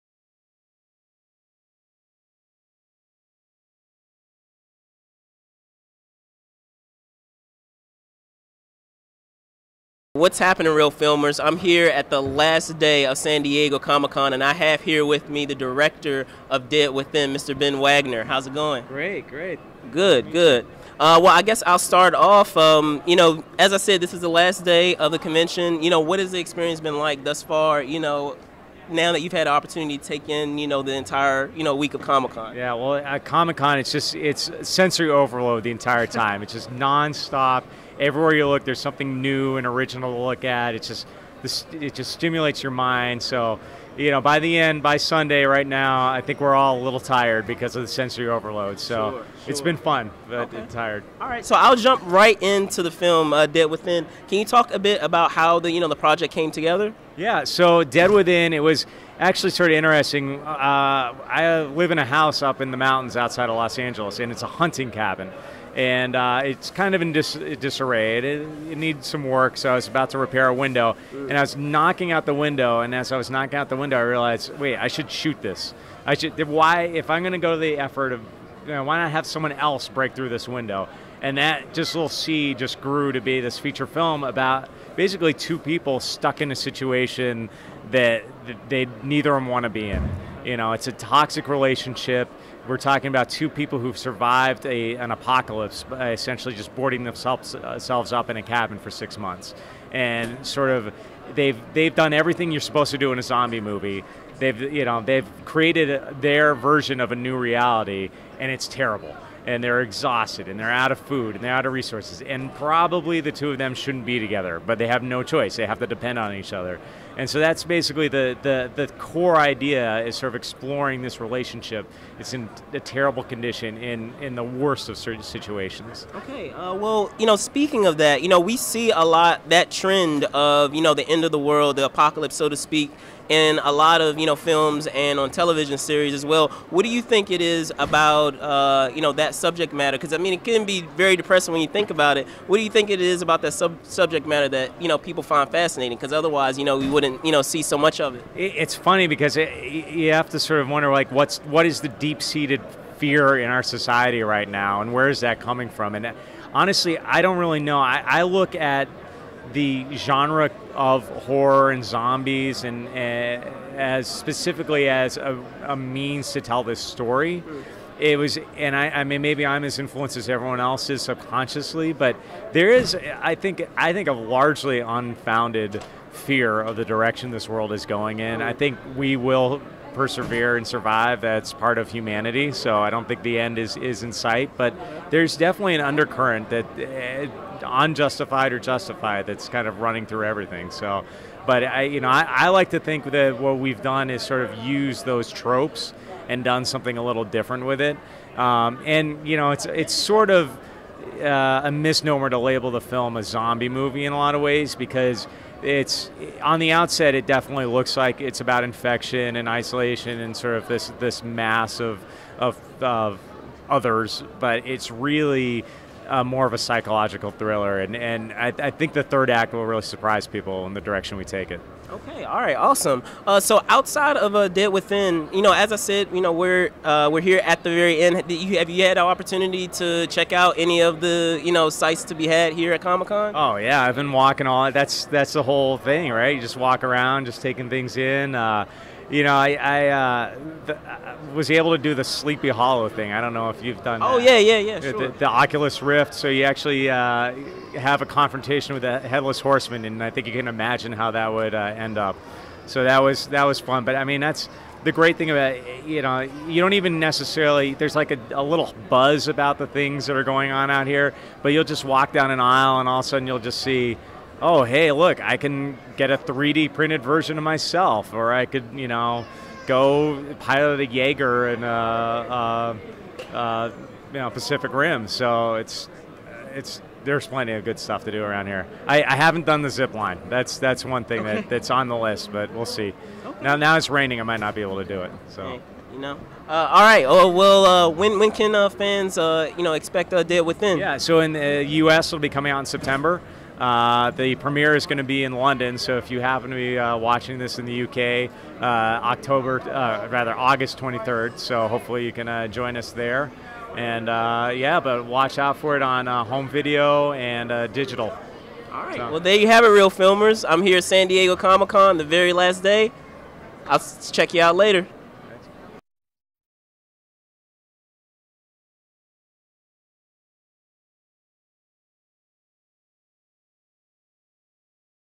SDCC 2014 EXCLUSIVE:Writer/Director Ben Wagner Talks DEAD WITHIN
Entertainment Informant Brandon Troy chats with writer/director Ben Wagner about his thriller, DEAD WITHIN.  In the interview, Mr. Wagner not only reveals what inspired the film, but also shares his insight as to why the subject matter addressed in the film seems to be so popular with others films and television.

Edited by Brandon Troy.